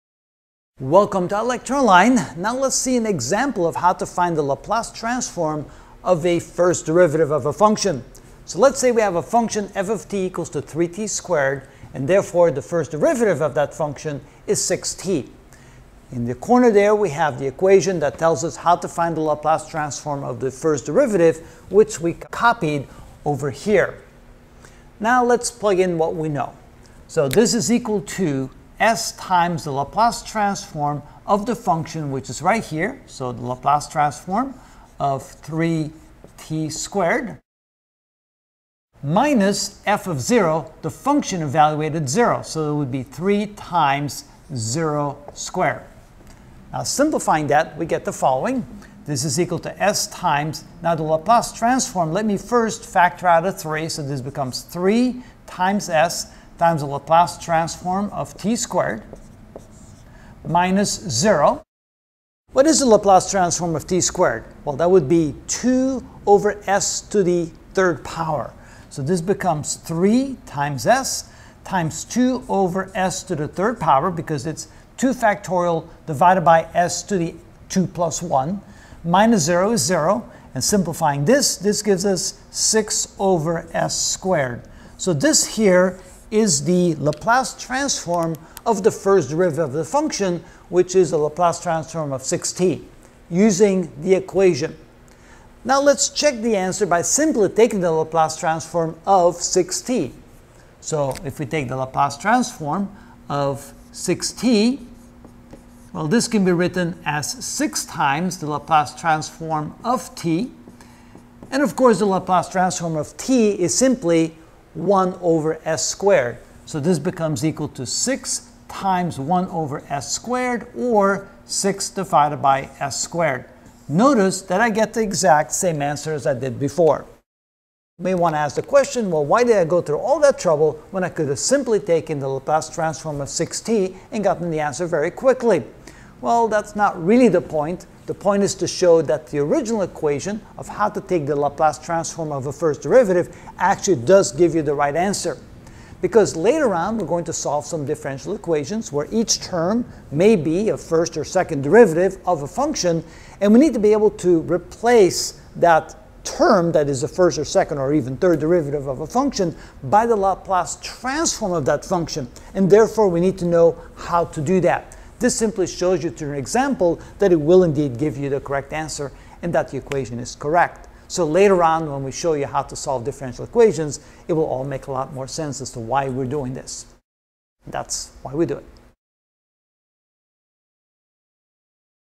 Electrical Engineering: Ch 16: Laplace Transform (21 of 58) Laplace Transf of the 1st Derivative: 1
In this video I will find the Laplace transform of the 1st derivative, L[f'(t)]=?, when f(t)=3t^2 and f'(t)=6t.